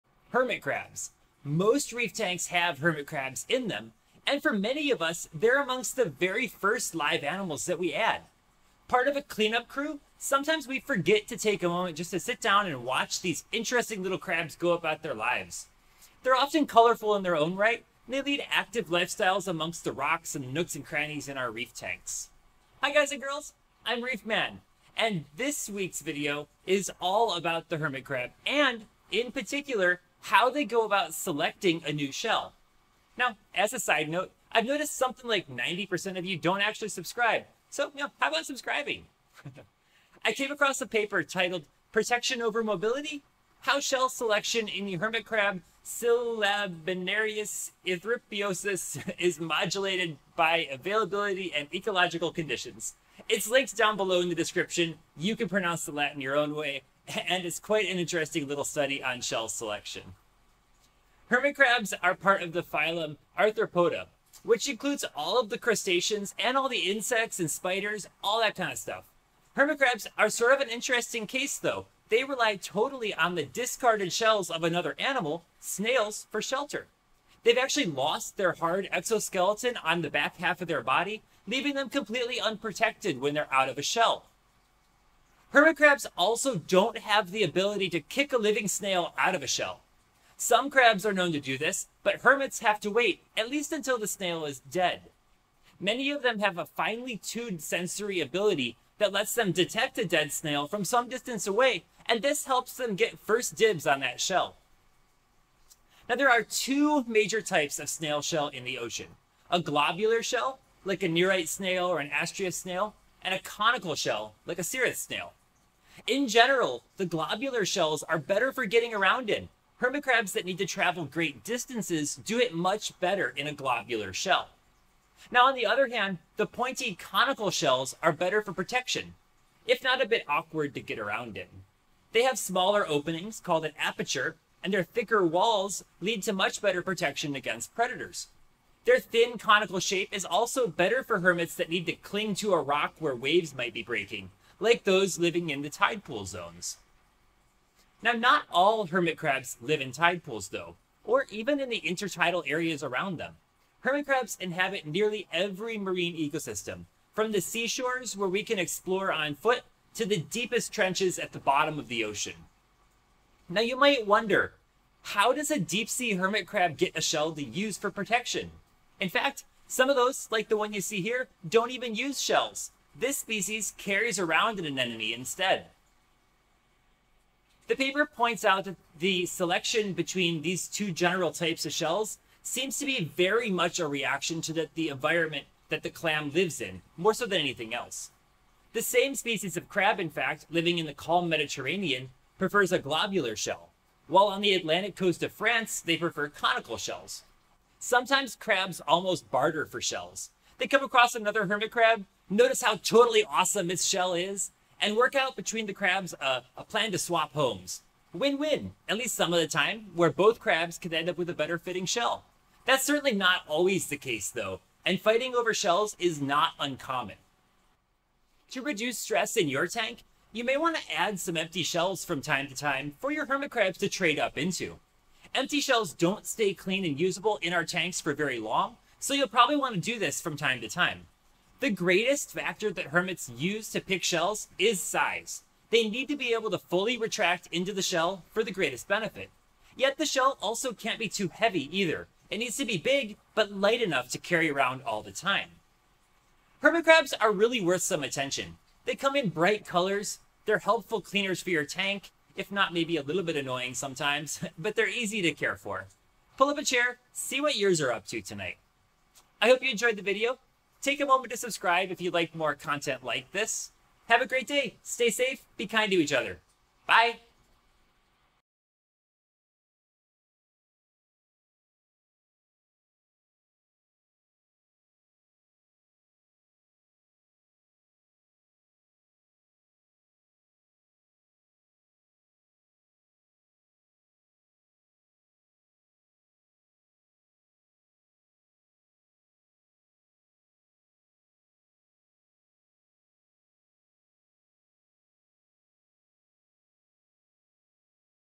Hermit Crabs and their Shells
The shell a hermit crab picks can tell you a lot about where it lives. Long, pointy shells are better for waves and protection from predators, but they're hard to get around in. More rounded shells are easier to get around in, but don't offer as much protection. Some hermits don't even use shells - there are deep sea hermit crabs that carry around anemones instead!

Take a moment to enjoy the goings on in your reef tank beyond the fish. Blue legged, Red legged, Halloween,  Zebra, there are hermit crabs in nearly every color. They're a very common part of our clean up crew, eating algae and left over food - yet sometimes they also steal food from LPS coral.

Protection over mobility? How shell selection in the hermit crab Clibanarius erythropus (Paguroidea) is modulated by availability and ecological conditions. Lena Elisa Dressler,  Roy Ebel, Sarah Ehlers.

The photo of a hermit crab out of it's shell is by Arnstein Rønning, and you can find it here: